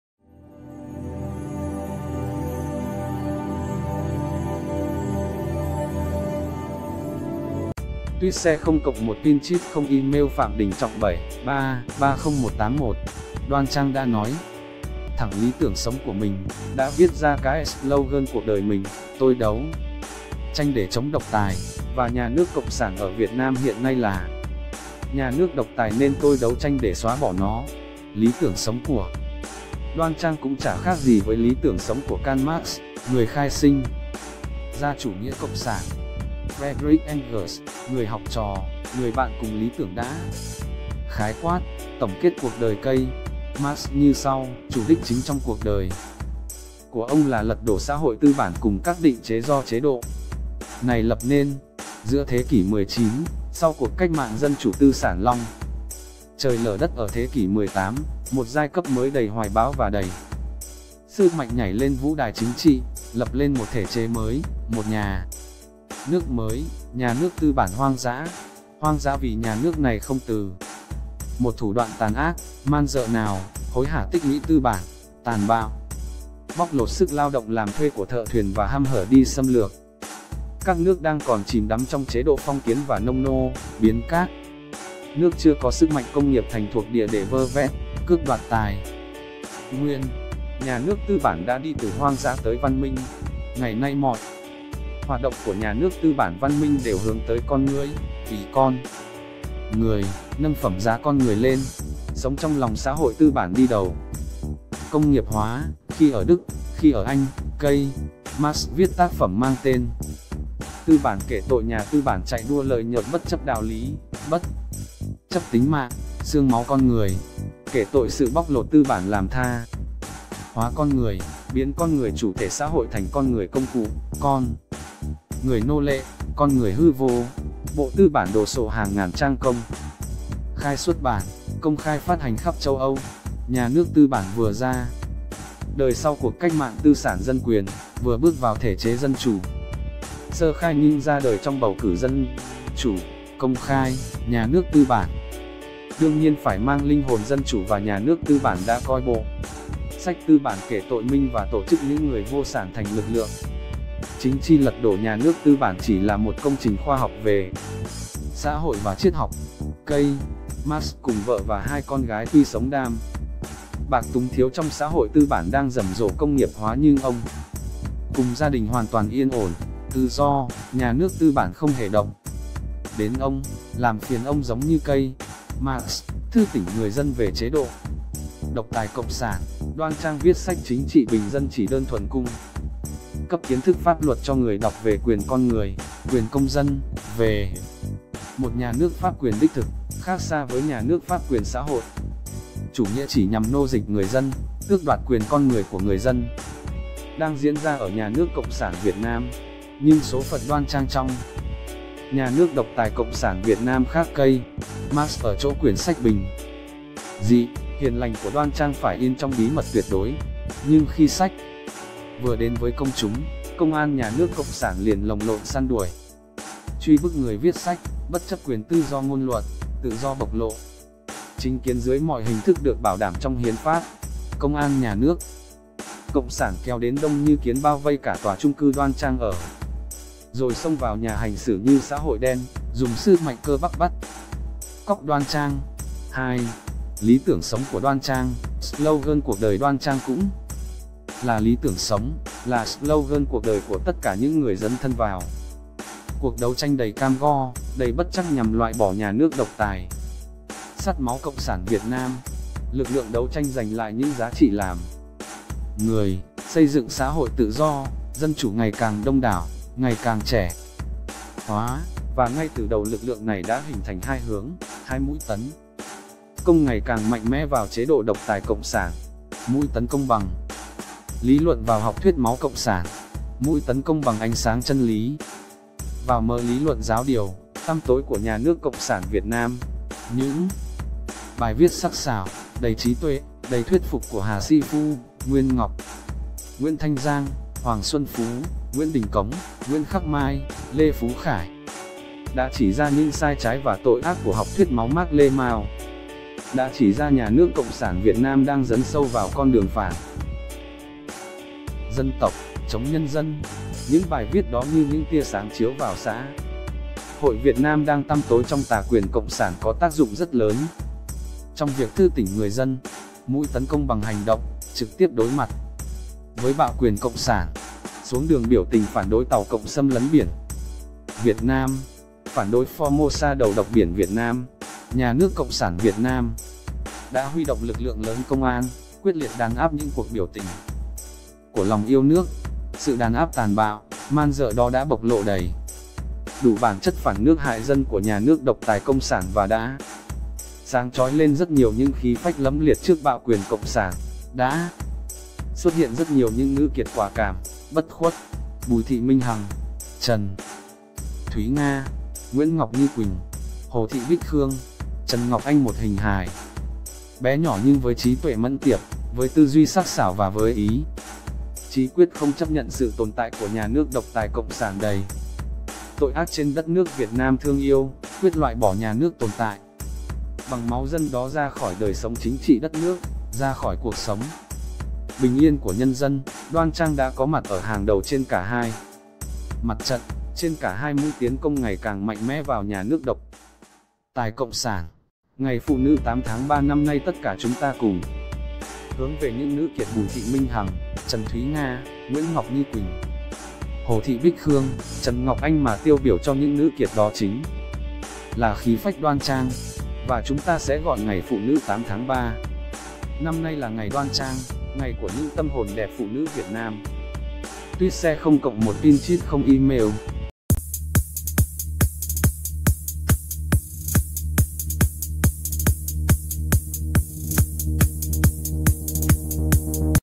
Ngày phụ nữ mồng 8 tháng 3 năm nay mang tên: Ngày Đoan Trang
Tweet Share 0 +1 Pinterest 0 Email Phạm Đình Trọng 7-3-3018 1. Đoan Trang đã nói thẳng lí tưởng sống của mình, đã viết ra cái slogan cuộc đời mình: Tôi đấu tranh để chống độc tài, và nhà nước cộng sản ở Việt Nam hiện nay là nhà nước độc tài nên tôi đấu tranh để xóa bỏ nó. Lí tưởng sống của Đoan Trang cũng chả khác gì với lí tưởng sống của Karl Marx, người khai sinh ra chủ nghĩa cộng sản. Fredrick Engels, người học trò, người bạn cùng lí tưởng đã khái quát, tổng kết cuộc đời K. Marx như sau: Chủ đích chính trong cuộc đời của ông là lật đổ xã hội tư bản cùng các định chế d...